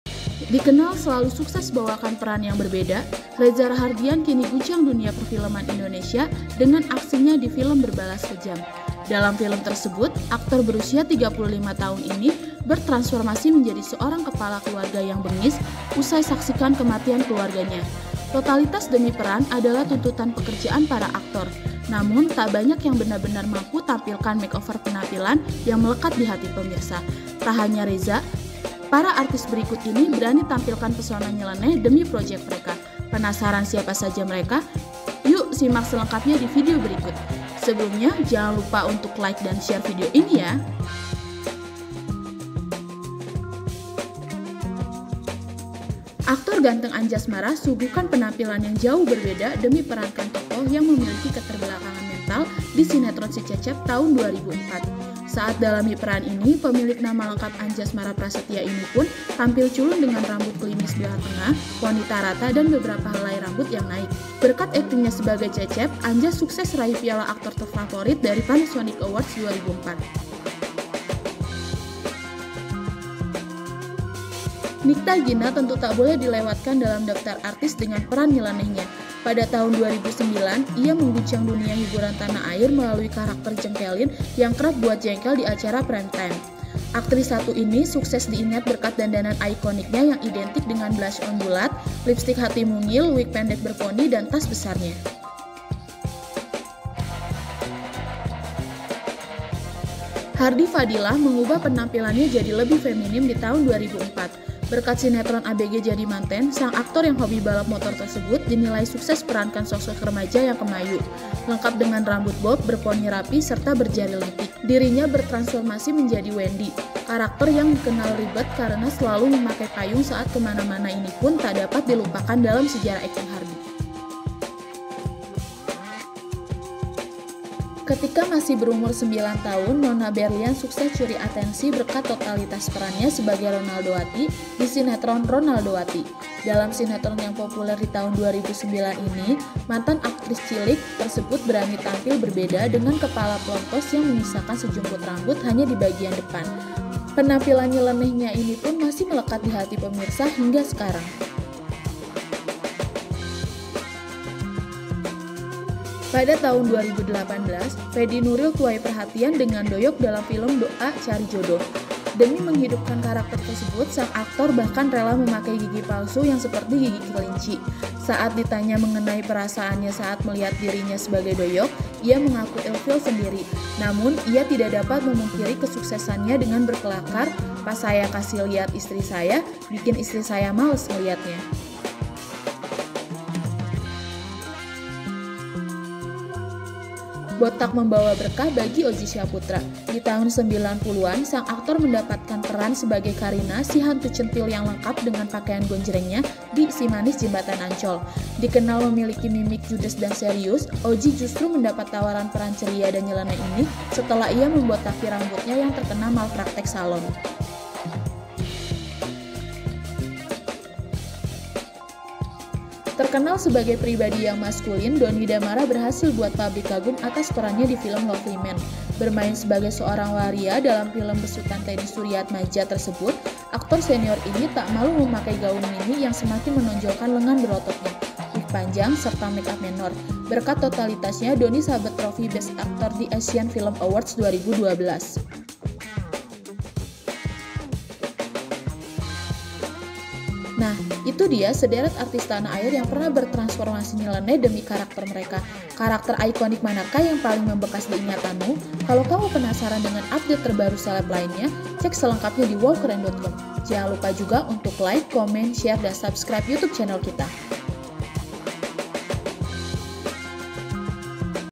Reza Rahadian Jadi Bad*ss di 'Berbalas Kejam', Sejumlah Artis Ini Malah Berubah Nyeleneh Demi Peran
Dikenal selalu sukses bawakan peran yang berbeda, Reza Rahadian kini guncang dunia perfilman Indonesia dengan aksinya di film "Berbalas Kejam". Dalam film tersebut, aktor berusia 35 tahun ini bertransformasi menjadi seorang kepala keluarga yang bengis usai saksikan kematian keluarganya. Totalitas demi peran adalah tuntutan pekerjaan para aktor. Namun, tak banyak yang benar-benar mampu tampilkan make over penampilan yang melekat di hati pemirsa. Tak hanya Reza, para artis berikut ini berani tampilkan pesona nyeleneh demi project mereka. Penasaran siapa saja mereka?